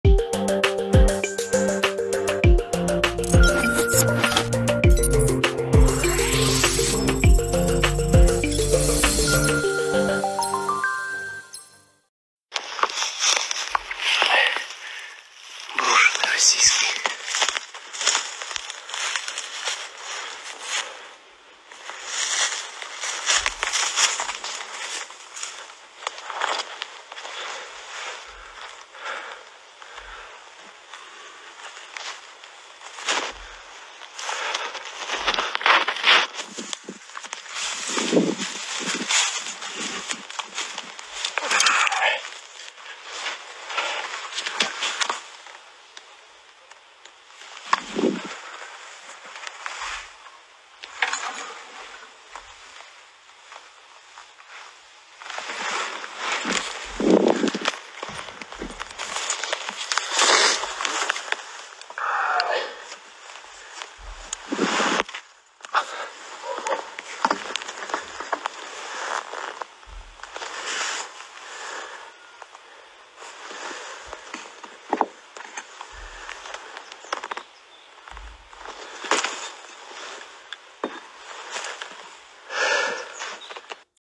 investigating an abandoned russian tank
INCREDABLE FOOTAGE OF THE INSIDE OF A RUSSIAN TANK AT WAR TIME.